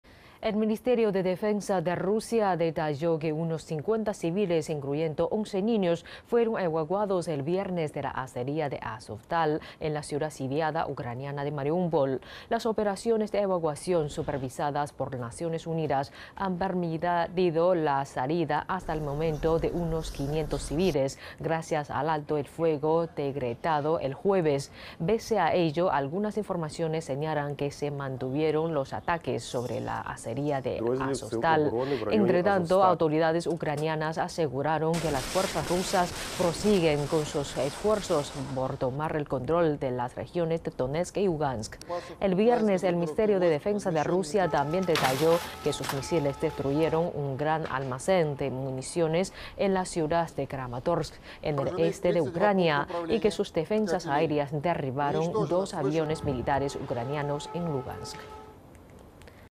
Prosigue la evacuación de civiles en la ciudad sitiada ucraniana de Mariúpol
El Ministerio de Defensa de Rusia detalló que unos 50 civiles, incluyendo 11 niños, fueron evacuados el viernes de la acería de Azovstal, en la ciudad sitiada ucraniana de Mariúpol.Las operaciones de evacuación, supervisadas por Naciones Unidas, han permitido la salida, hasta el momento, de unos 500 civiles, gracias al alto el fuego decretado el jueves. Pese a ello, algunas informaciones señalan que se mantuvieron los ataques sobre la acería de Azovstal. Entre tanto, autoridades ucranianas aseguraron que las fuerzas rusas prosiguen con sus esfuerzos por tomar el control de las regiones de Donetsk y Lugansk. El viernes, el Ministerio de Defensa de Rusia también detalló que sus misiles destruyeron un gran almacén de municiones en la ciudad de Kramatorsk, en el este de Ucrania, y que sus defensas aéreas derribaron dos aviones militares ucranianos en Lugansk.